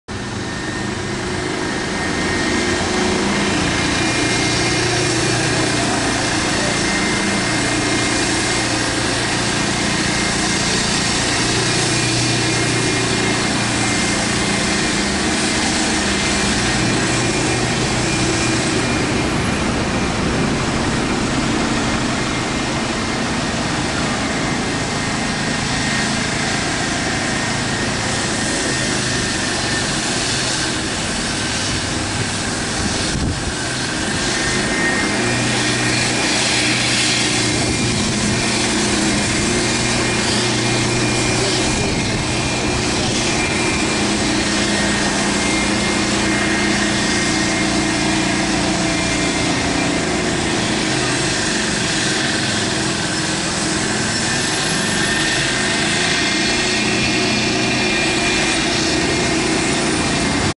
Tameside Don't do what these two women did at Ashton Bus Station you could get run over by a bus
Tameside Don't do what these two women did at Ashton Bus Station it's dangerous you could get run over by a bus Ashton Bus Station Ashton Under Lyne